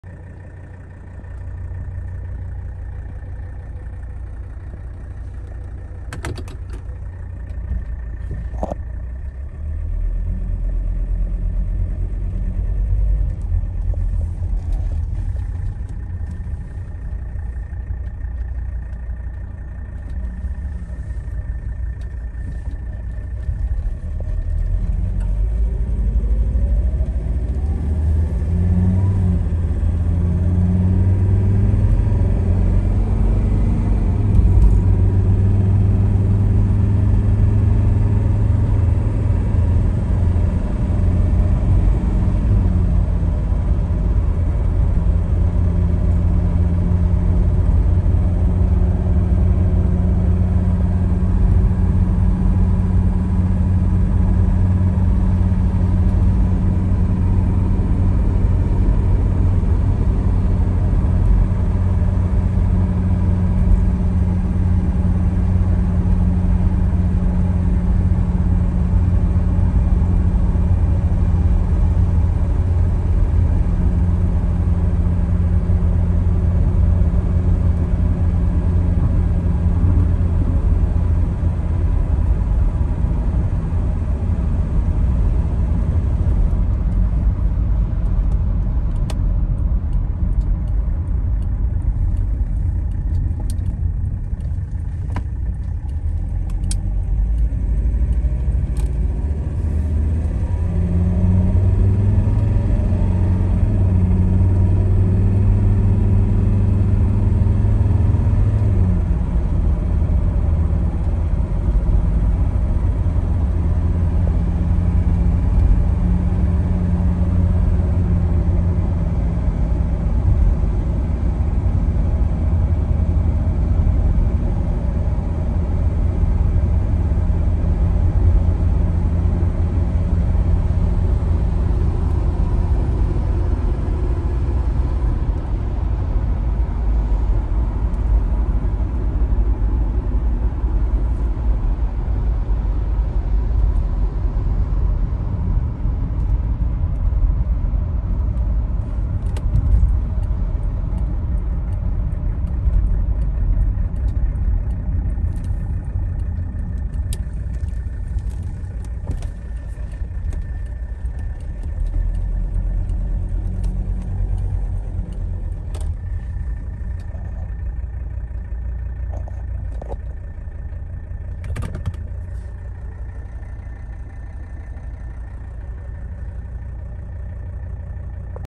1985 Mercedes-Benz 300TDT Wagon Driving: The MB Market
Bid for your chance to own this 1985 Mercedes-Benz 300TDT Wagon  only on The MB Market!

The MB Market is the premier auction platform specifically for Mercedes-Benz vehicles and parts. Bid now on TheMBmarket.com!